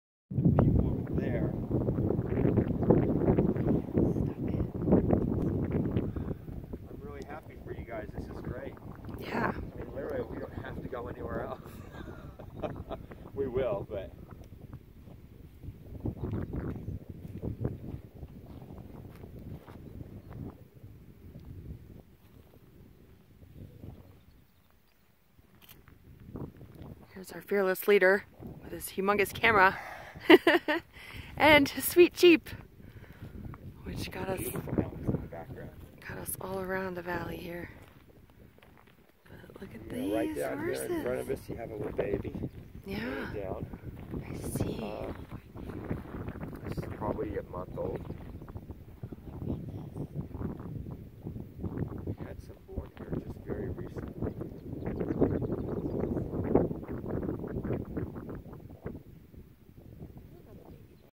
Wild Horses in Carson Valley, Nevada
Dwayne's World takes us on a Jeep tour to discover wild bands of horses in Carson Valley, Nevada.